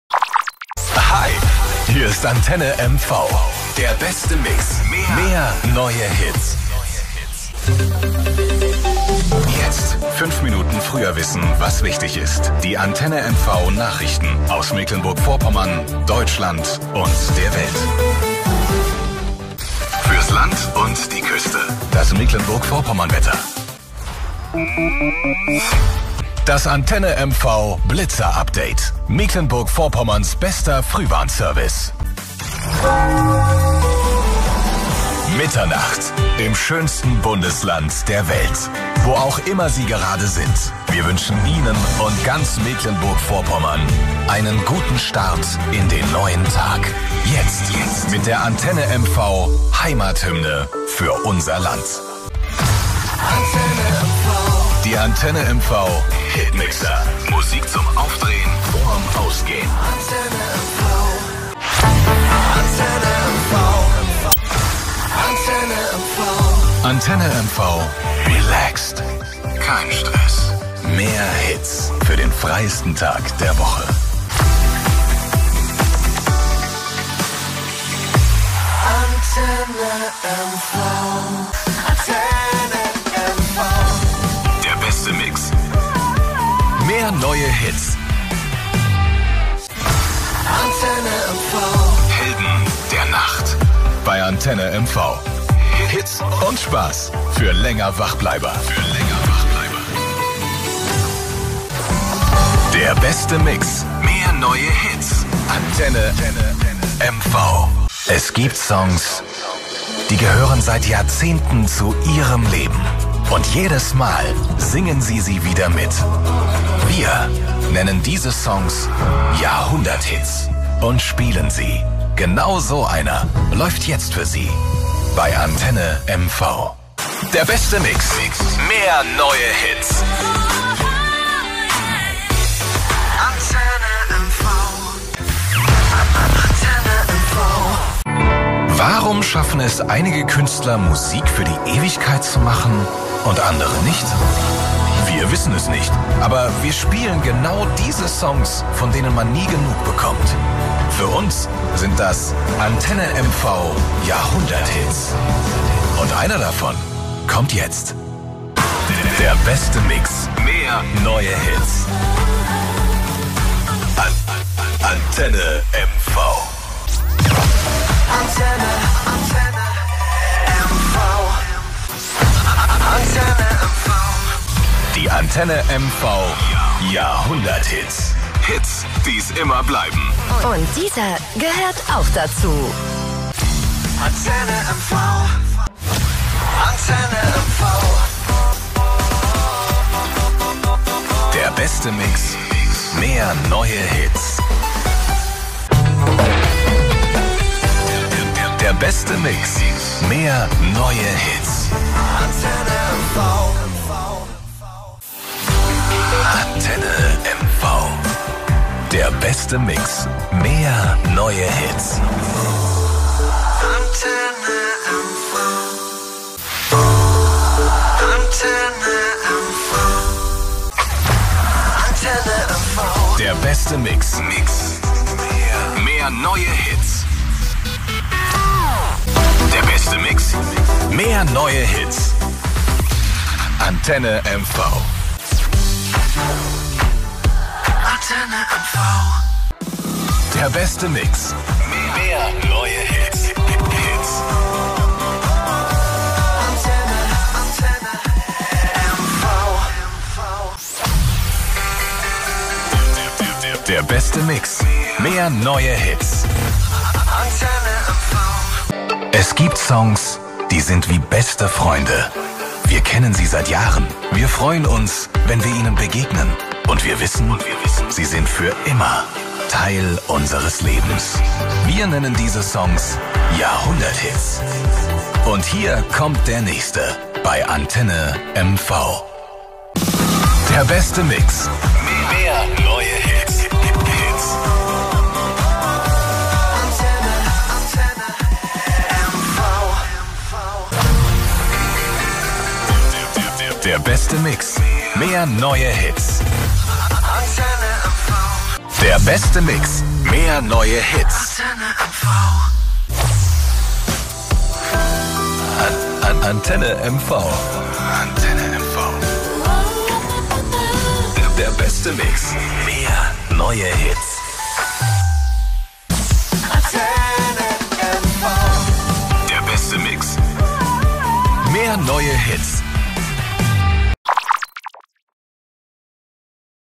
Antenne MV - Jingles (2014)
Antenne MV Jingles 2014 (Update mit neuem Sound und neuer Stationvoice)

Antenne MV - Der beste Mix. Mehr neue Hits
REGIOCAST | Radioservices (2014)